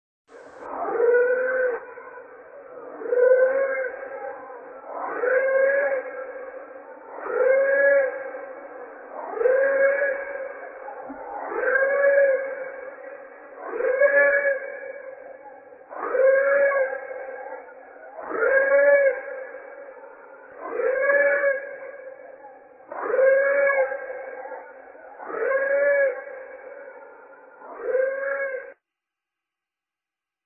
How I think dinosaurs sound like
Birds are descendants of dinosaurs ain't they? If we want to know how dinosaurs used to make themselves heard, our best bet should be based on bird callings. With that idea in mind, I lowered the speed and pitch of a recording of the Channel-billed Cuckoo (Scythrops Novaehollandiae), a bird of Australia. The result is... quite stimulating.